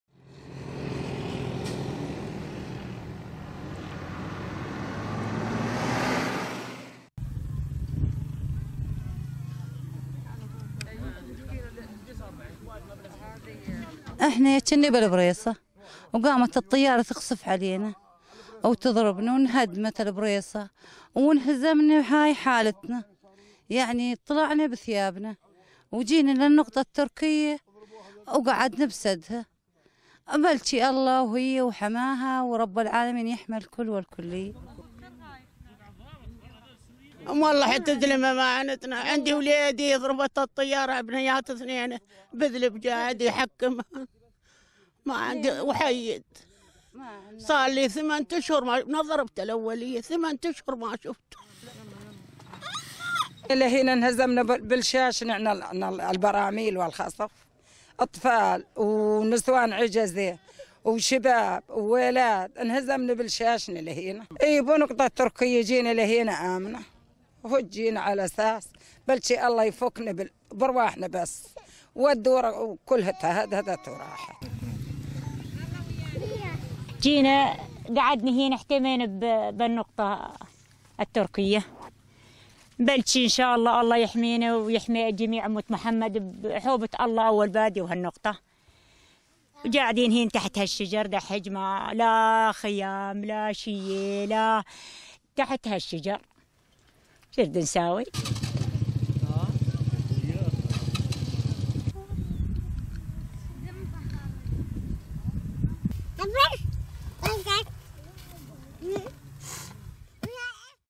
نزوح أهالي ريف إدلب إلى محيط نقاط المراقبة التركية
شهدت منطقة ريف إدلب حركة نزوح كثيفة باتجاه الأراضي الزراعية ونقطة المراقبة التركية نتيجة استهداف مليشيا أسد الطائفية تلك المنطقة بالبراميل المتفجرة والقذائف الصاروخية وأقاموا خيامهم بجانب تلك النقطة بحثا عن الأمان.
تغطية: محمد الفيصل